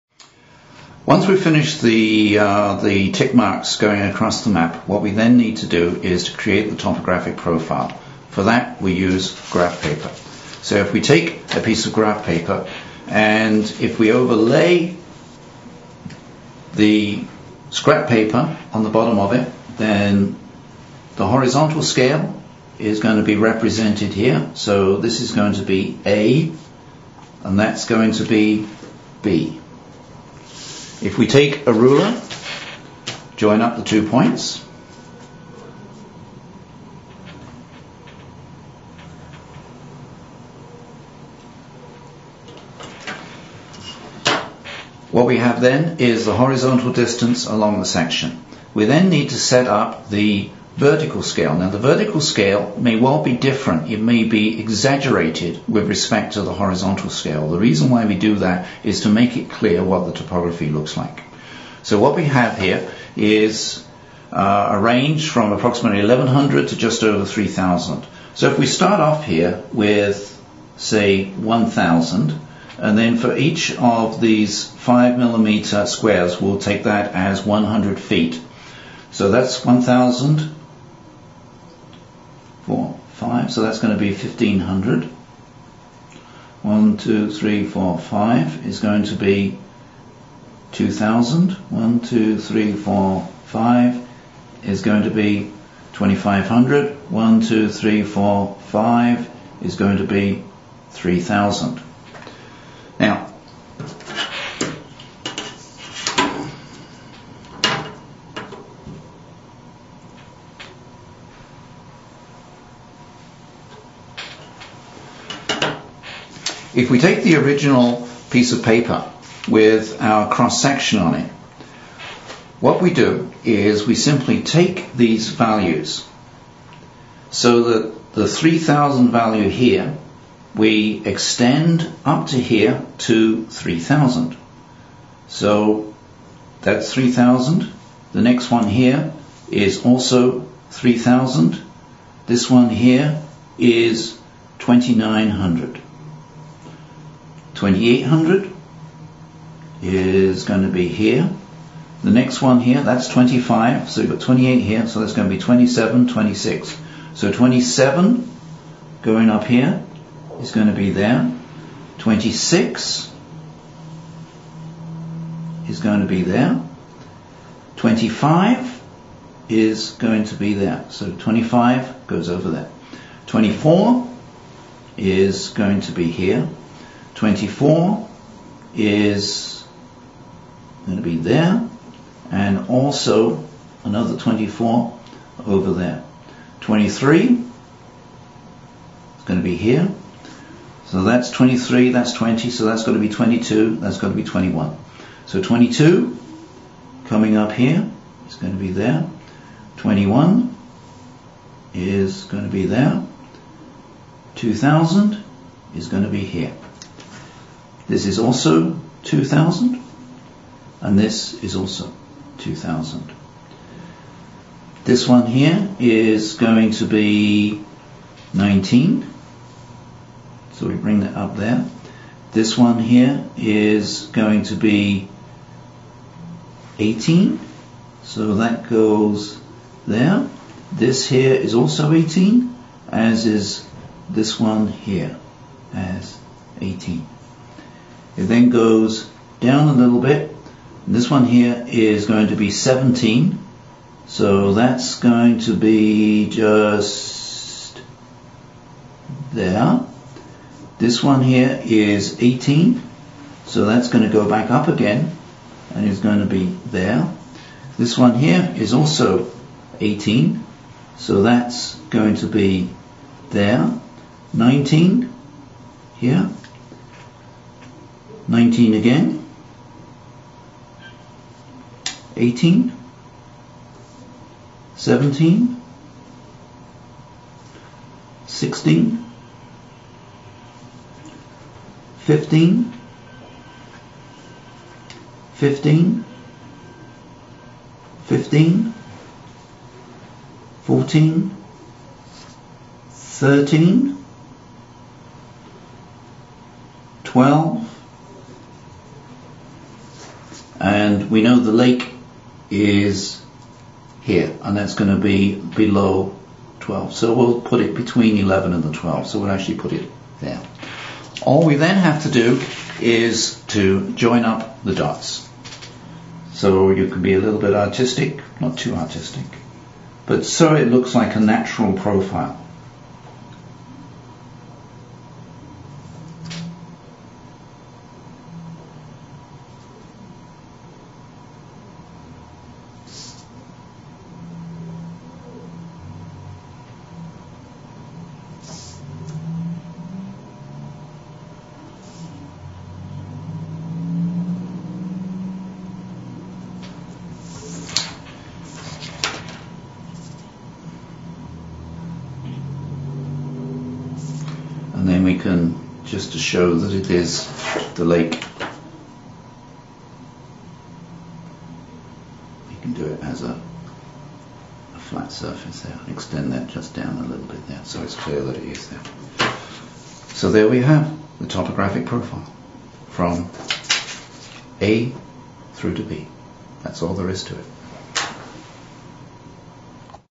Topographic Map Profiles - Part 2
demonstration of how to create a topographic map profile